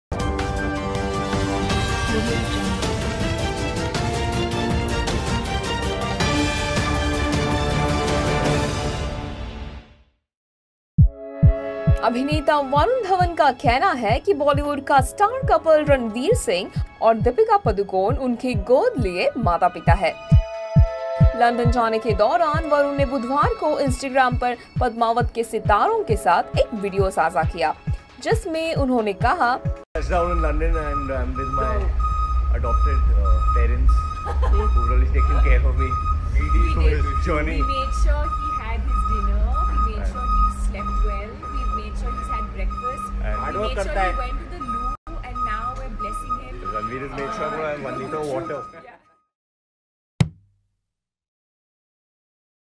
Varun Dhawan 'Adopts' Deepika Padukone, Ranveer Singh as Parents
Actor Varun Dhawan says Bollywood star couple Ranveer Singh and Deepika Padukone are his "adopted" parents.On his way to London, Varun on Wednesday shared a video along with the 'Padmaavat' stars on Instagram and was heard saying: "...I am with my adopted parents. Ther are taking care of me in this journey..."